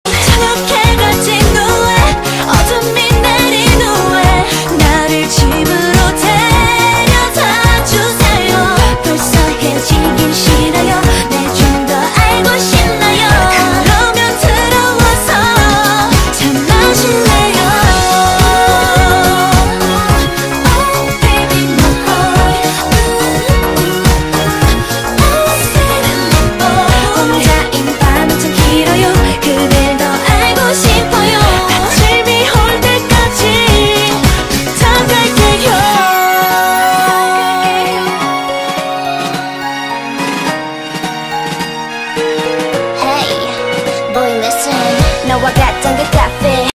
A SAUDADE DAS HELLO VENUS JUNTAS 🥲
Pra quem não sabe, a Nara, uma das minhas atrizes favoritas e que fez Suspicious Partner, Itaewon Class, Bulgasal, entre outros dramas, também era do grupo Hello Venus! Talento redobrado! 😉❤️

🎤 Grupo: Hello Venus